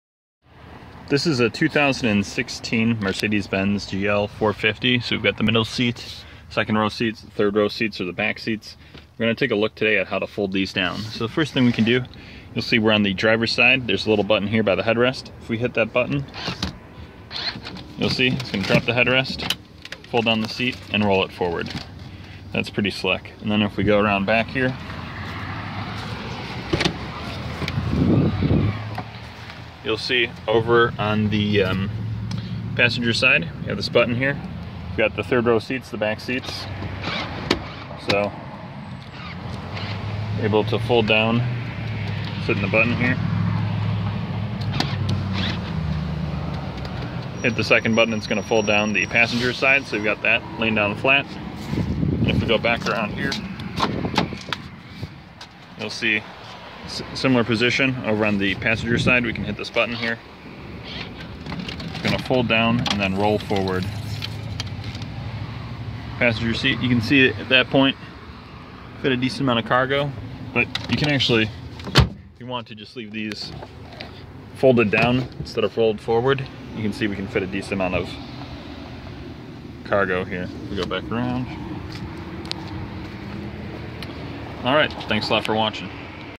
Folding Seats on a 2016 Mercedes Benz GL450
This video shows how to fold down the seats on a Mercedes Benz GL450.